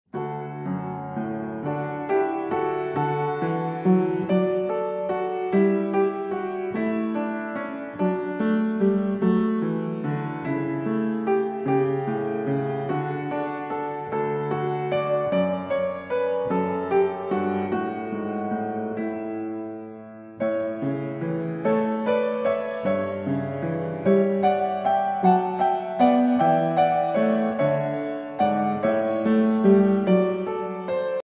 A flatworm under a microscope.
This flatworm ate so much that it changed its morphology, getting a unique shape similar to a dinosaur. An important aspect to consider when identifying in the microcosm, many organisms can change in shape beyond recognition... Viewed under a 40x magnification.